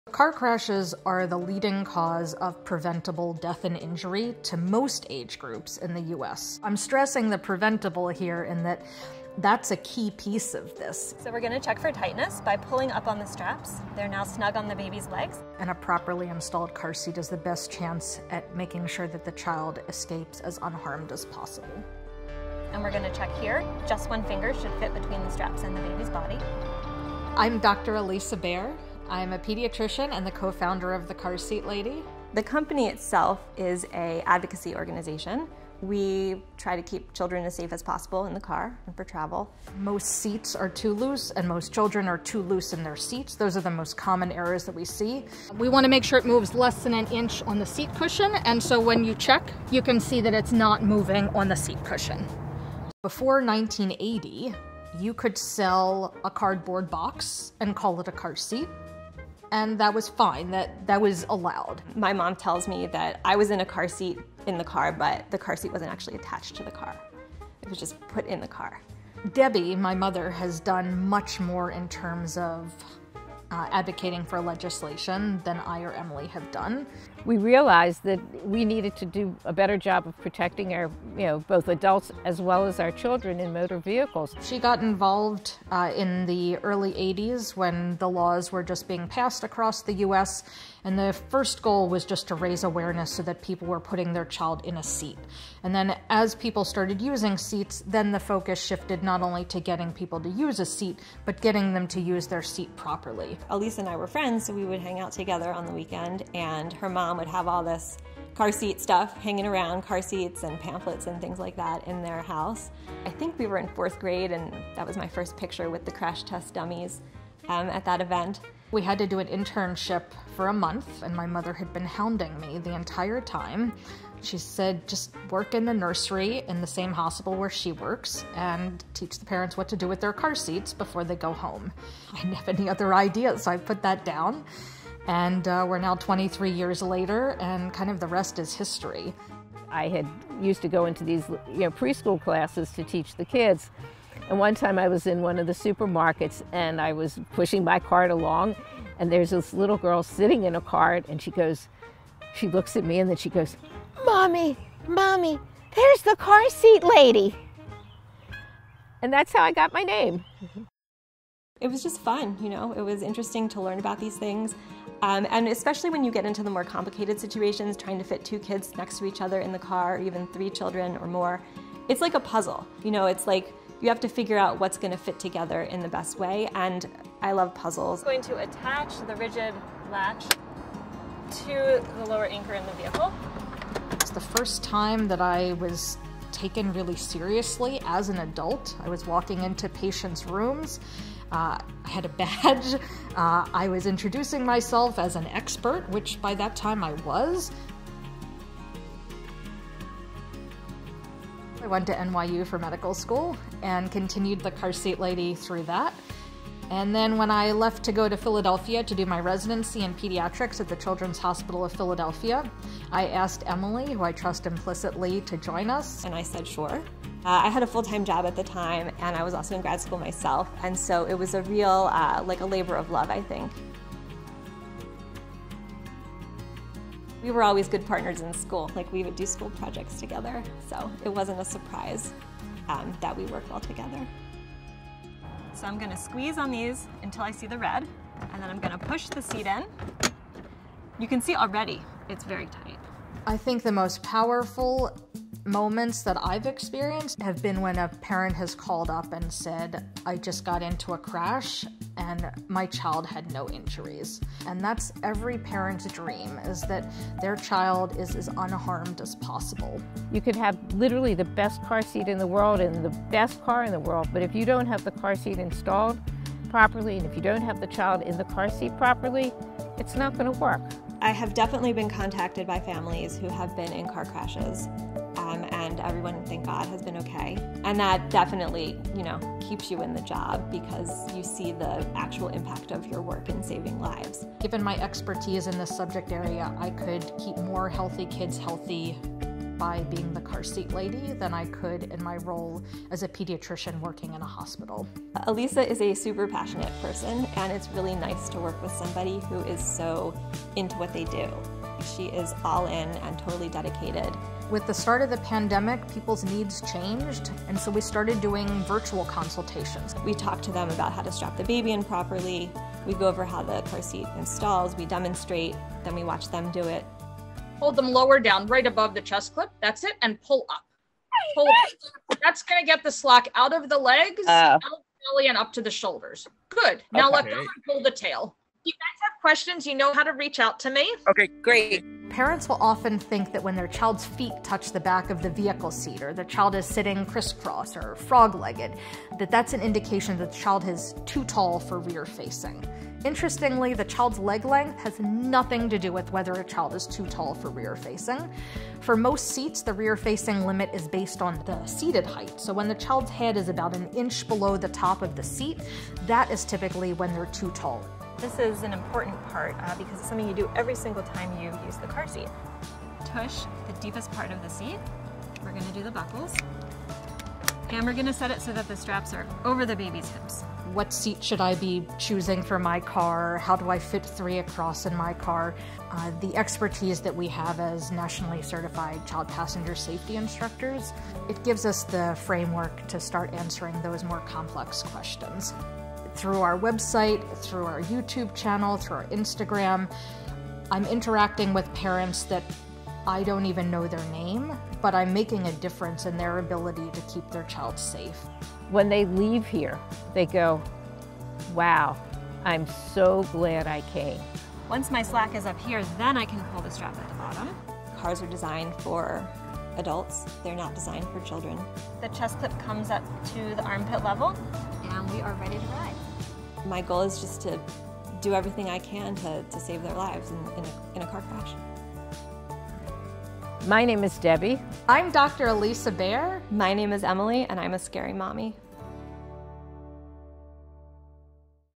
I Am Scary Mommy | The Car Seat Lady: Keeping Children Safe for Generations
The Car Seat Lady has been keeping kids safe in seats since WE were kids. This level of car seat knowledge will blow your mind.